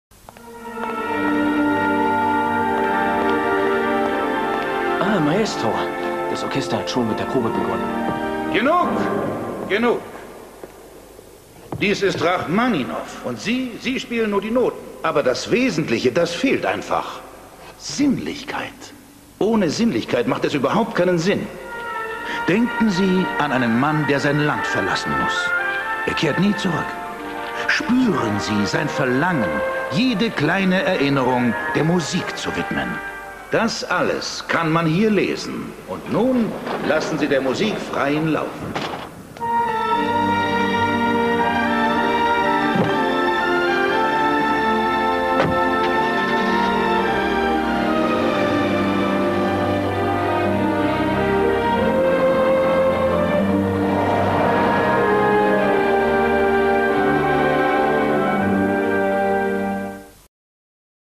ClassicalMusicInAds MERCEDES BENZ
Anuncio perteneciente al Calendario de Adviento 2023 "Música Clásica en los Anuncios de TV" Actividad para fomentar la escucha de Música Clásica.